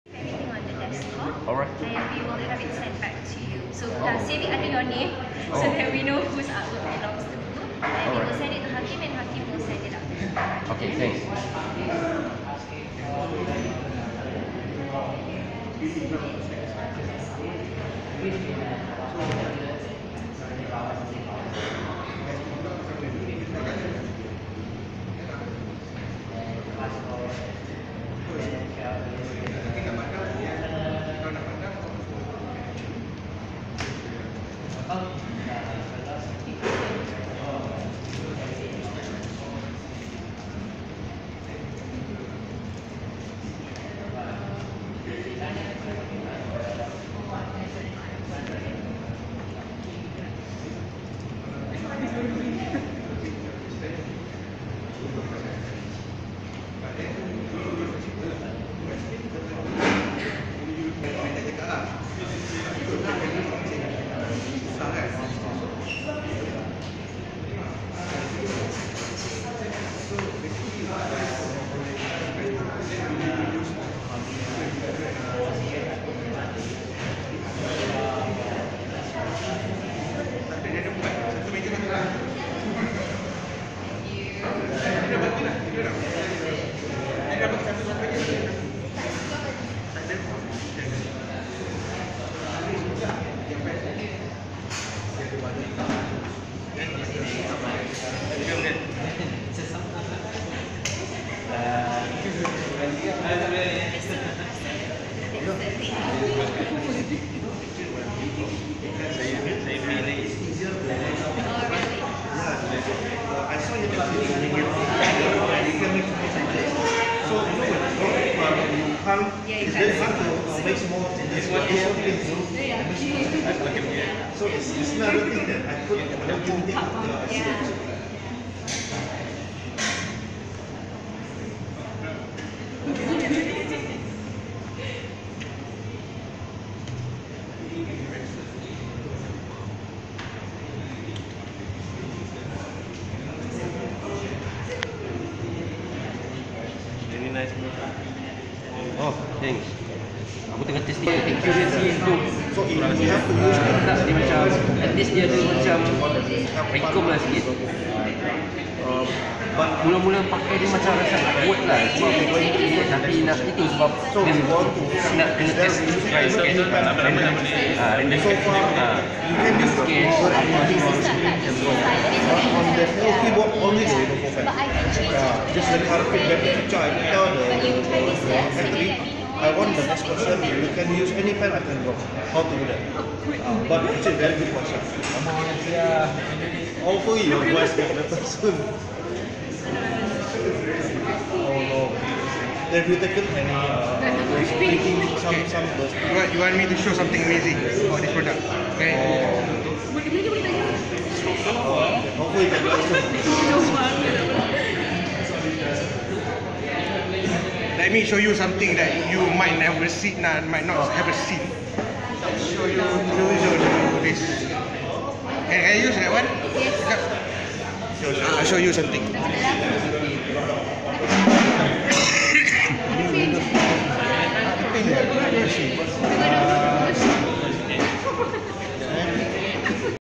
Bengkel Lenovo Yoga Book Bersama KL SketchNation - Part 3
Seni lakaran ini dihasilkan oleh Syamsul PS a.k.a. Skullhellustrasion. Lakaran yang berkonsepkan animasi dan komik ini sungguh mudah dan tidak menggunakan apa-apa kertas lakaran langsung. Hanya menggunakan Real Pen dan drawing pad yang terdapat pada peranti Yoga Book ini.